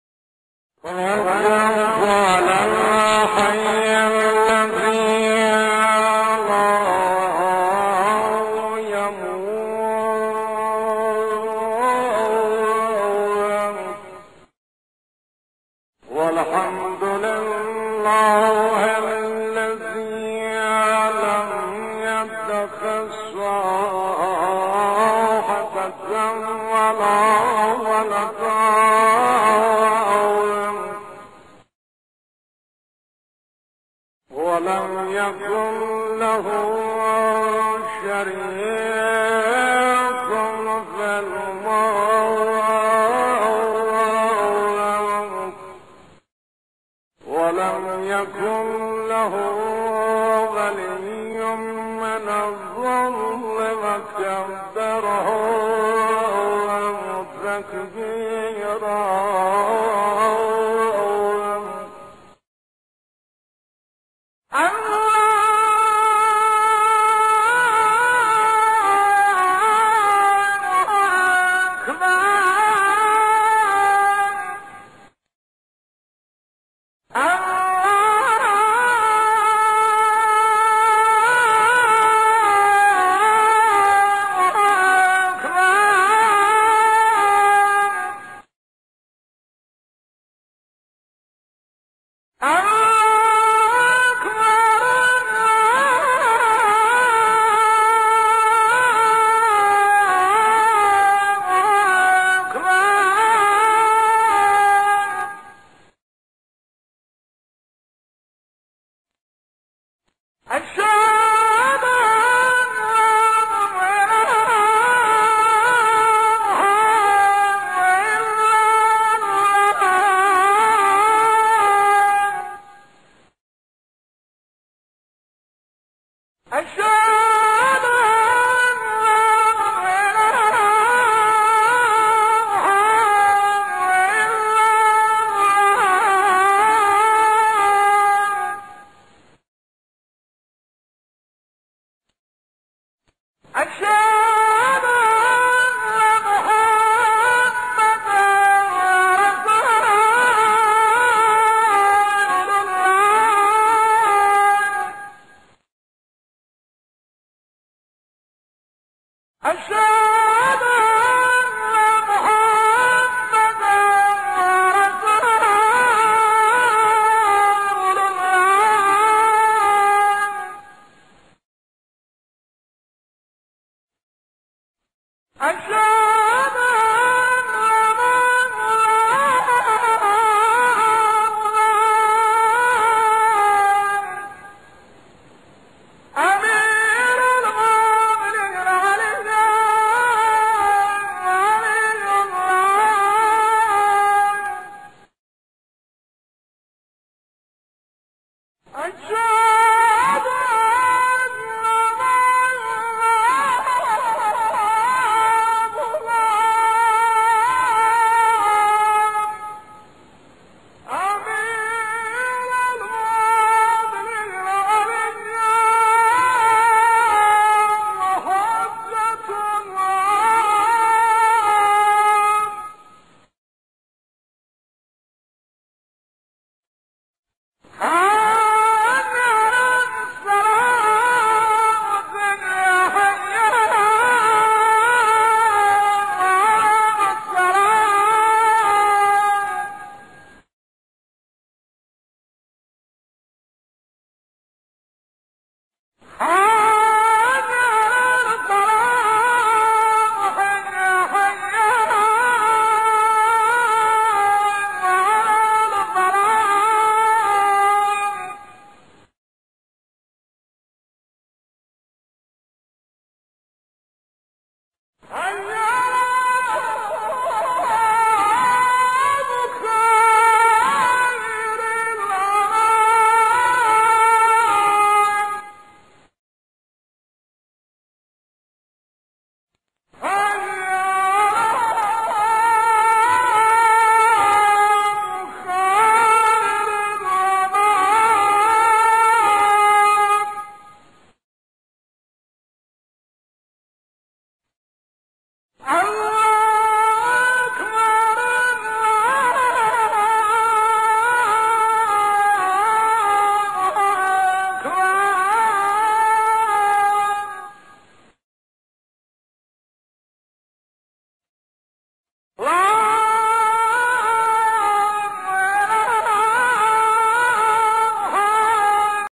'Şia Ezanı' Üstad Müezzinzade Ezan
Bu sese hasret kalanlara..

Tevekkeltü alel hayyillezi la yemutü ebeden

isra 111

vel hamdü lillahillezi lem yettehiz veleden velem yekün lehü şerikün fil mülki ve lem yekün lehü veliyyün minezzülli ve kebbirhü tekbira

Abdulbaki Gölpınarlı Meali
isra 111
Ve de ki: Hamd Allah'a ki oğul edinmemiştir kendisine ve saltanatta, tasarrufta ortağı yoktur ve aciz olmadığından yardımcıya da ihtiyacı yoktur ve pek büyük bil, onu, büyüklüğünü de bildir.